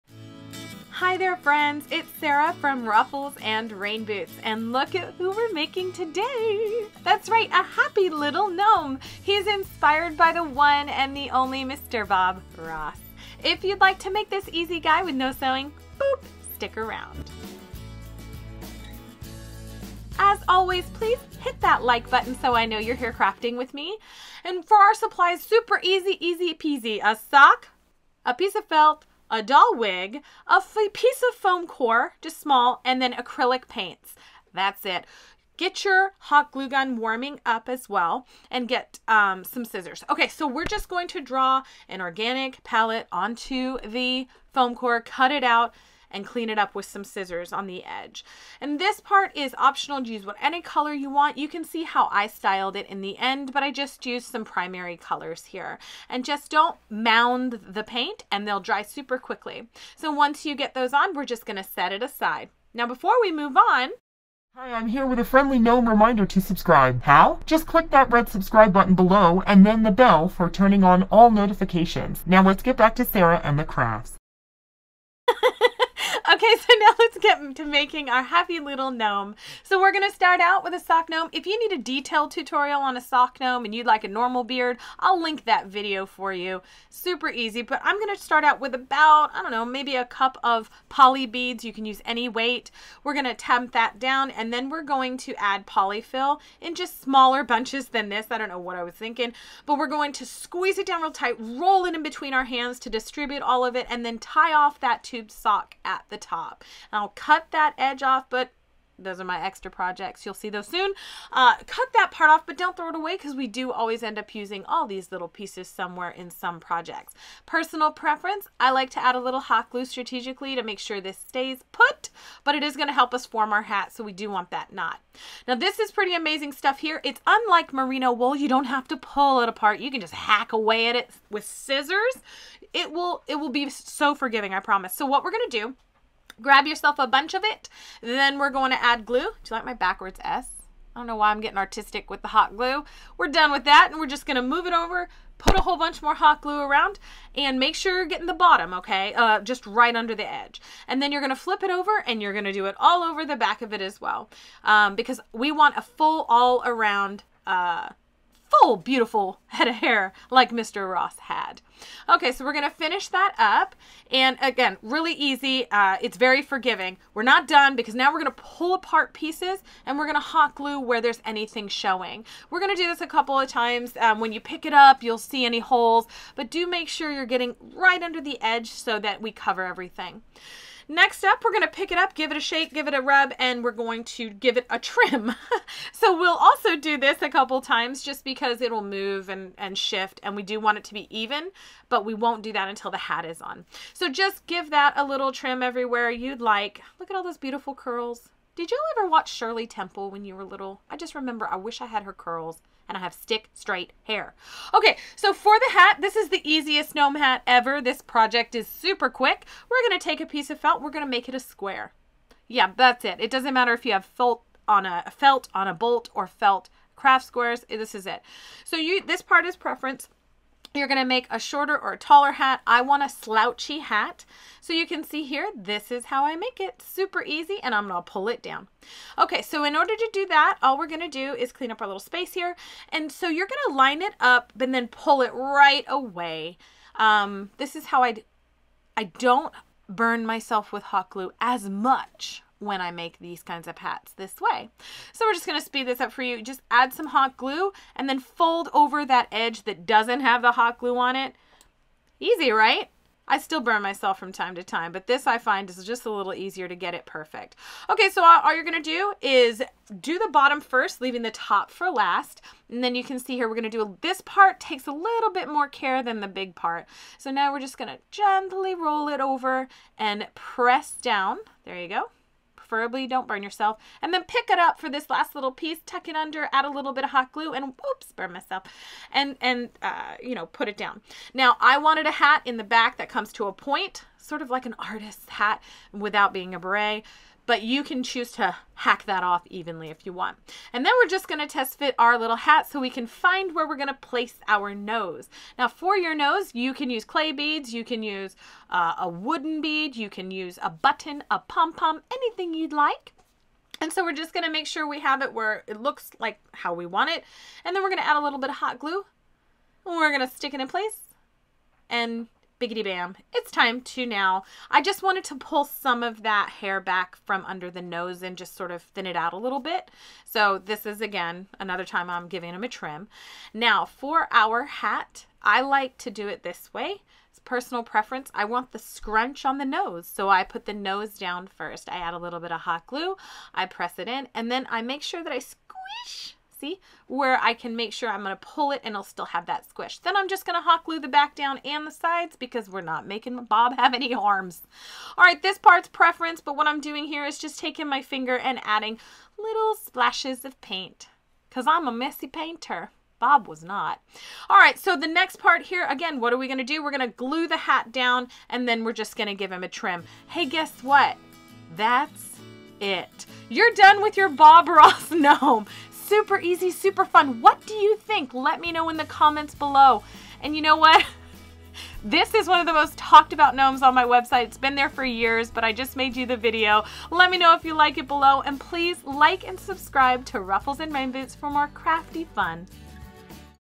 Bob Ross Gnome 5 Minute Sock Gnome
Some of you might have seen this little Bob Ross Gnome years ago on my website (Ruffles and Rain Boots), but I made a video after getting so many requests to make him. Note: there are a LOT of differences, so do check out the original at the link below for inspiration.

Did you know he is a 5-minute sock gnome?! And the best part is that the gnome hat pattern - if you can even call it that - is done in about 45 seconds... I'm not kidding, this is one of the easiest gnomes to make yourself.

You can grab the "hair" here*

Psst: I used to be able to find the tiny wood palette at the craft stores but cannot anymore, so I also share how to make an artist's palette.

For the original post and tips on making this artist gnome,

Note: the original "How to Make a Sock Gnome" video is here. In it, I detail how to work with faux fur gnome beards, how to make a sock gnome, how to make a gnome hat from a sock, and more.

Thank you for joining me. Be sure to connect and stay in touch: